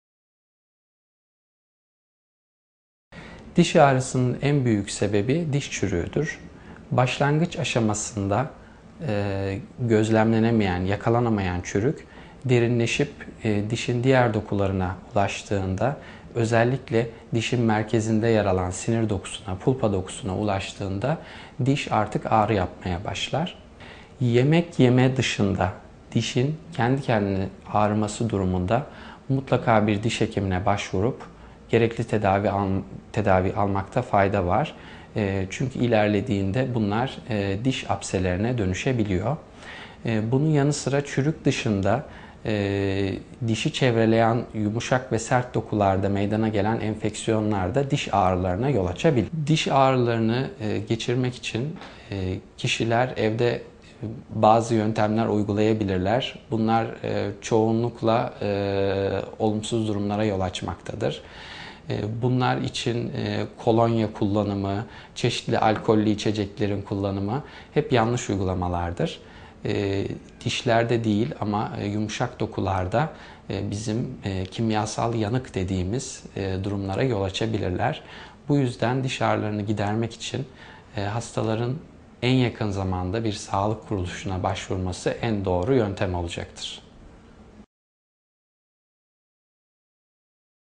Diş Ağrısı
Evde diş ağrılarını geçirmek için neler yapılmalıdır?